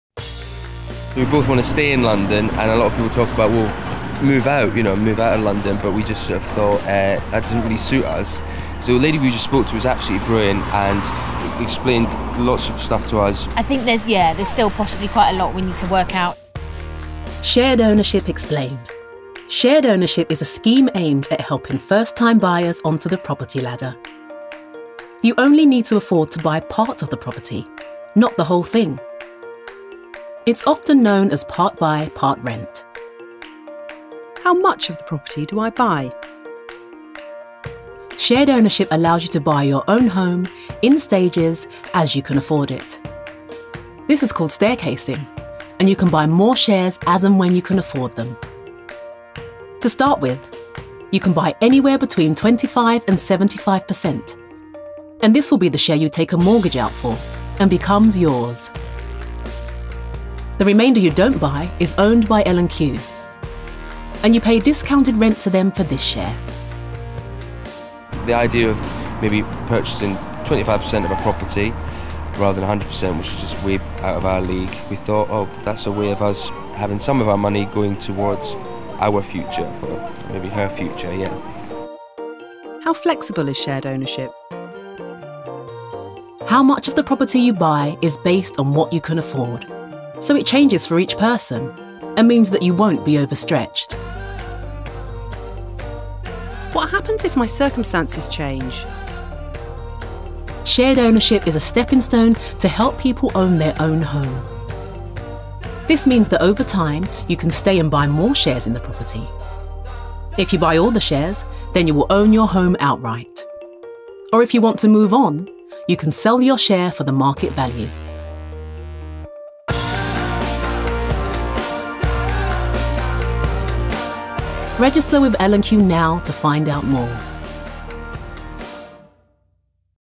What is Shared Ownership?
Shared Ownership makes it possible to own your home – even if you feel priced out of the property market. It’s an accessible home ownership scheme designed as a stepping-stone to outright ownership if you can’t afford to buy a home on the open market.